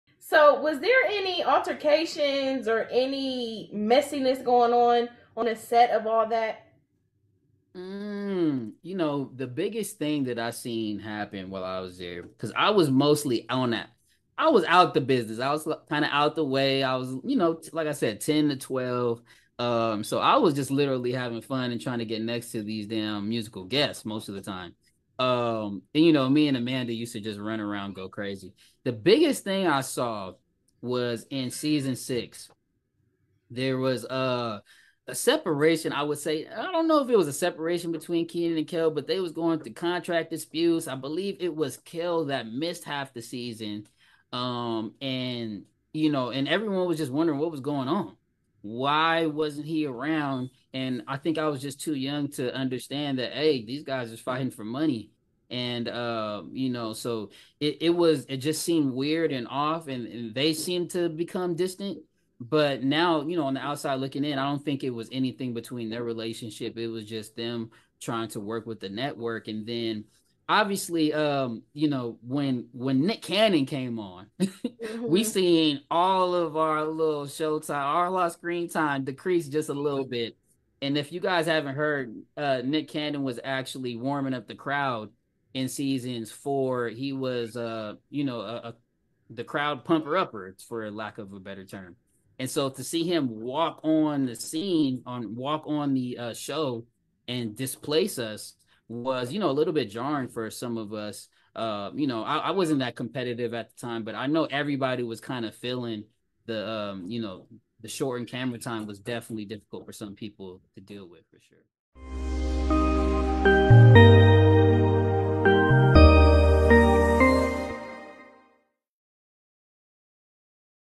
Leon “Leeboy” Frierson On Backstage Drama While On The Set Of All That
Quia & Kevie ask Leon about any possible altercations while he was apart of the All That cast.